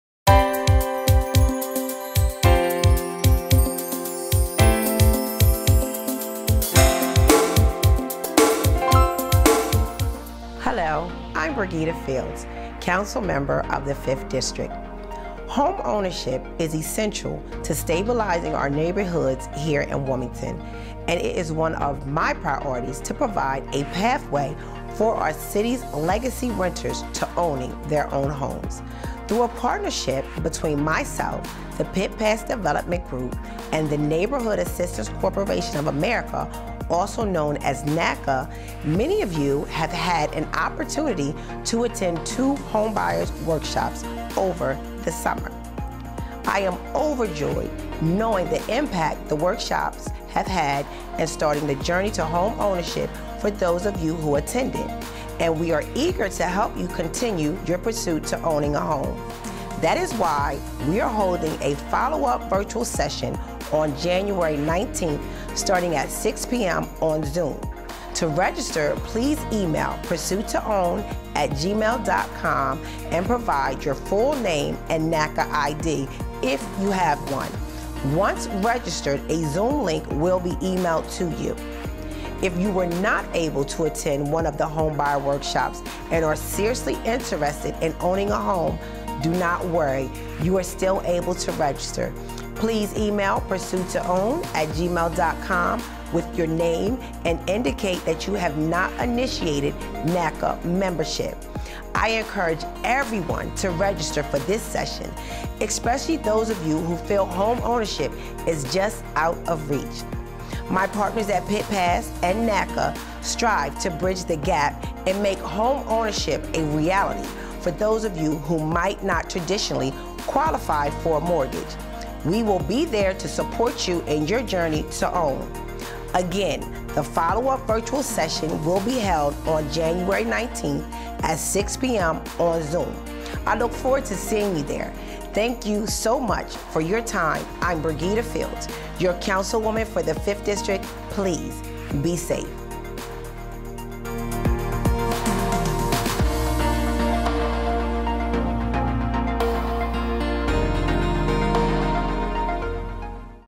Two Minute Drill | 5th District Council Member Bregetta Fields | December 30, 2022
In this Two Minute Drill, 5th District Council Member Bregetta Fields talks about an upcoming follow-up virtual session to the homeownership workshops that were held in partnership with PittPass Development Group and NACA at both the Williams “Hicks” Anderson Community Center and the Chase Center over the summer.

If you did not attend one of the original workshops, this virtual session is still open to anyone interested and serious about owning a home in Delaware.

The virtual follow-up session will be held on January 19, 2023 at 6:00pm on ZOOM.

Provide your full name and NACA ID if you have one, or if you have not initiated membership with NACA, include that in the e-mail. A ZOOM link will be provided once you have been registered.